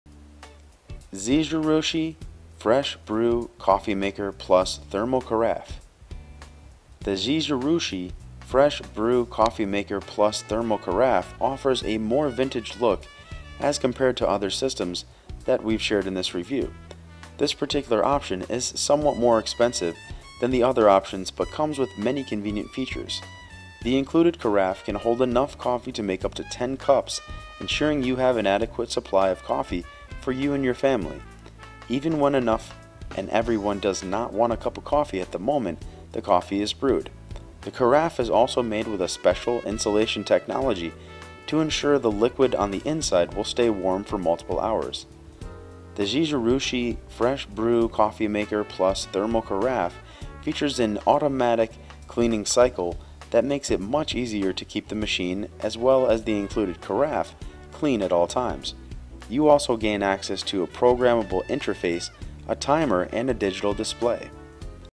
Zojirushi Fresh Brew Coffee Maker Plus Thermal Carafe Review
Zojirushi Fresh Brew Coffee Maker Plus Thermal Carafe

The Zojirushi Fresh Brew Coffee Maker Plus Thermal Carafe offers a more vintage look as compared to other systems that we have shared on our list. This particular option is somewhat more expensive than the other options but comes with many convenient features. The included carafe can hold enough coffee to make up to 10 cups, ensuring you have an adequate supply of coffee for you and your family, even when everyone does not want a cup of coffee at the moment the coffee is brewed. The carafe is also made with a special insulation technology to ensure the liquid on the inside will stay warm for multiple hours.

The Zojirushi Fresh Brew Coffee Maker Plus Thermal Carafe features an automatic cleaning cycle that makes it much easier to keep the machine, as well as the included carafe, clean all the time. You also gain access to a programmable interface, a timer, and a digital display.

Reviews of the 9 Best Thermal Carafe Coffee Makers, Plus 1 to Avoid